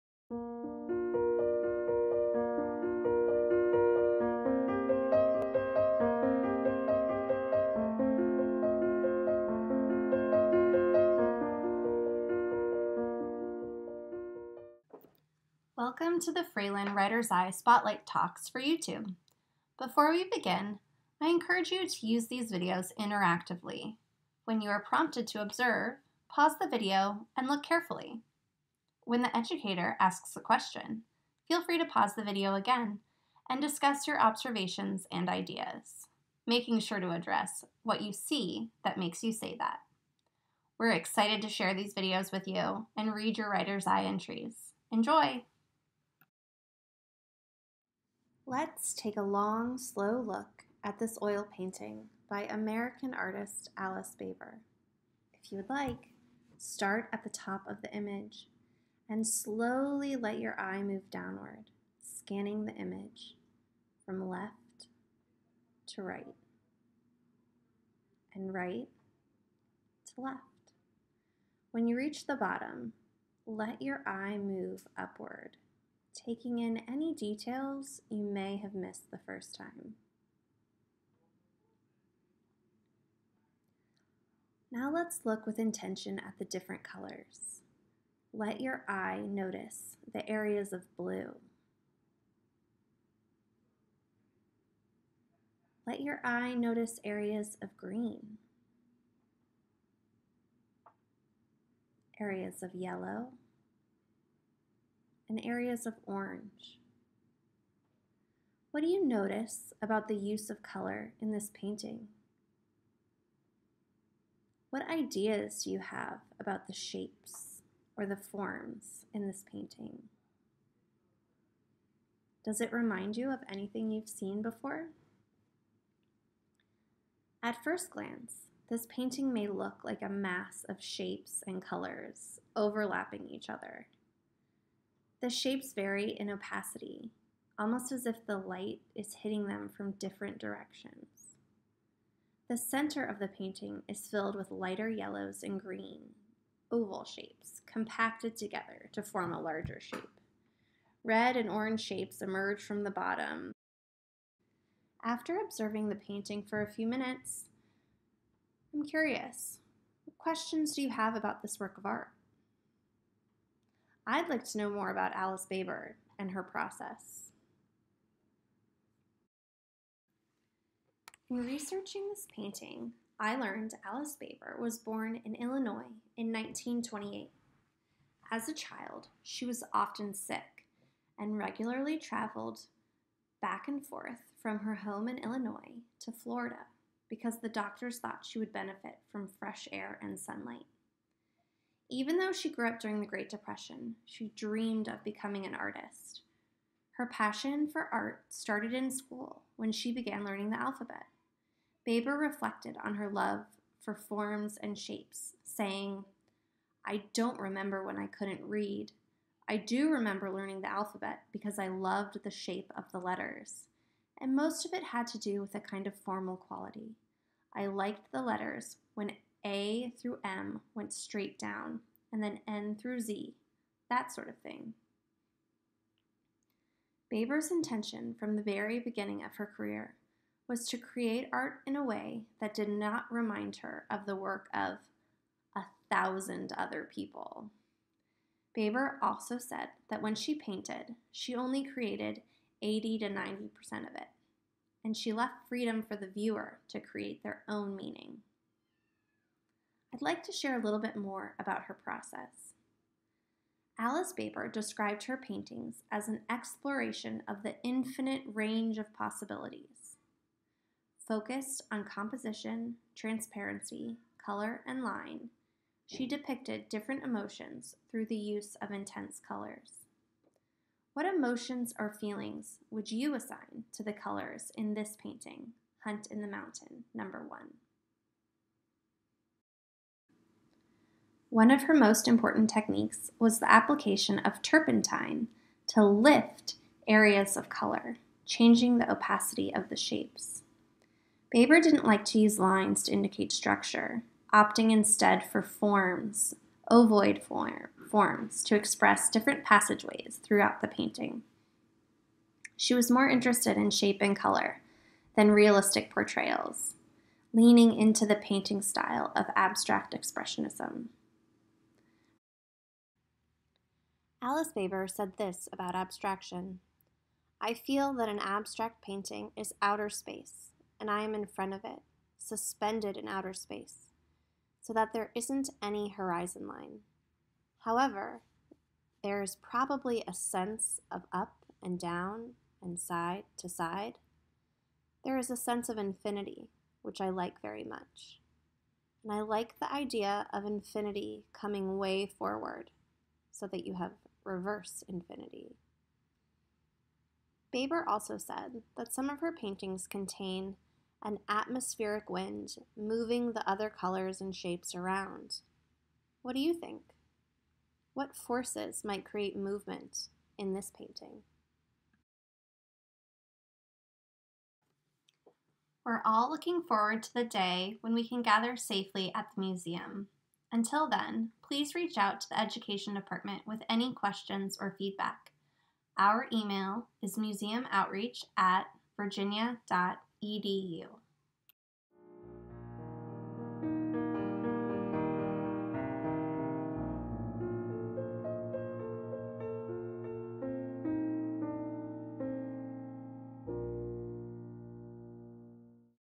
Writer’s Eye Spotlight Tour: Alice Baber’s "Hunt in the Mountain #1"
Experience Alice Baber’s  “Hunt in the Mountain #1” in this Spotlight Tour, created for Writer’s Eye 2020, The Fralin Museum of Art’s 34th annual ekphrastic writing competition. Fralin educator Emily Lazaro narrates guided viewing, and shares information about Baber—an artist who described herself as having “a thirst for color,” and said that her work contained “an atmospheric wind moving the other colors and shapes around.”

Alice Baber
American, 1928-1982

Hunt in the Mountain #1, 1972

Oil on canvas, 14 x 12 inches (35.6 x 30.5 cm)
The Fralin Museum of Art at the University of Virginia
Bequest of Buzz Miller. The Alan Groh-Buzz Miller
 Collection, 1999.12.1